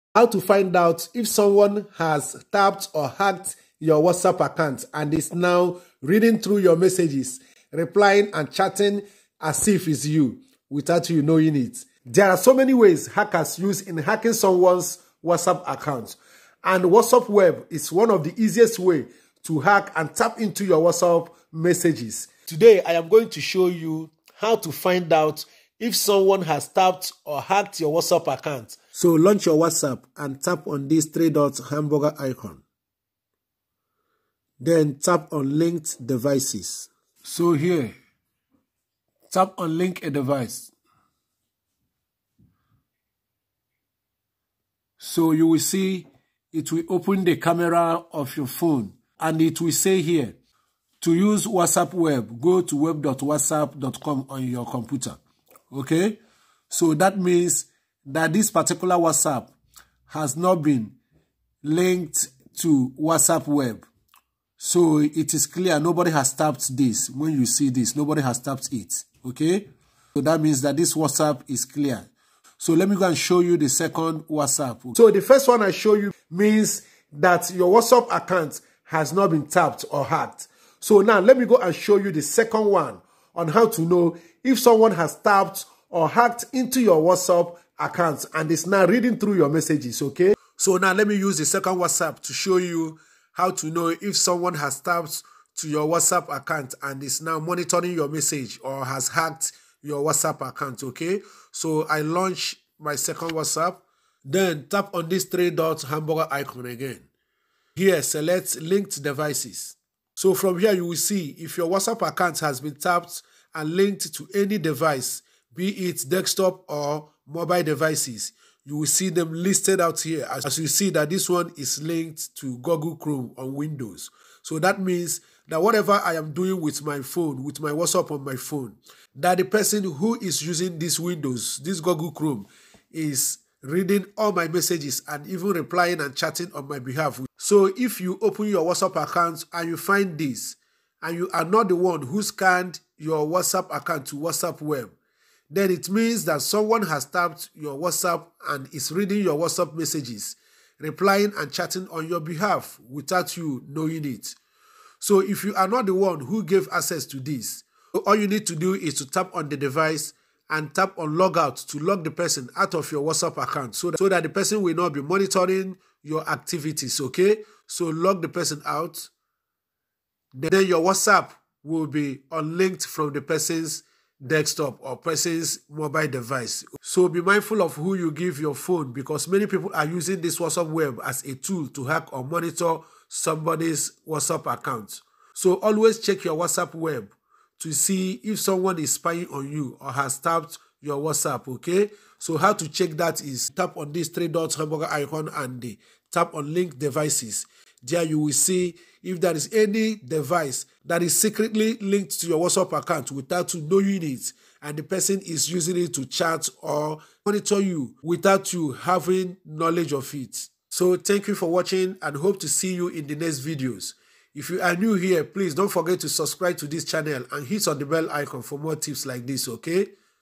How To find out if someone has Hacked Your WhatsApp Account
In this Video you will see how to find out if someone has tapped or hacked your WhatsApp Account Without your knowledge.

there are many ways to hack a WhatsApp Account and WhatsApp web is one of the easiest way to hack and Tap into someones WhatsApp messages.

so in this Video you will see how to find out if your WhatsApp has falling a victim to this trap.

If you need a video RGB Light for your video then check this out.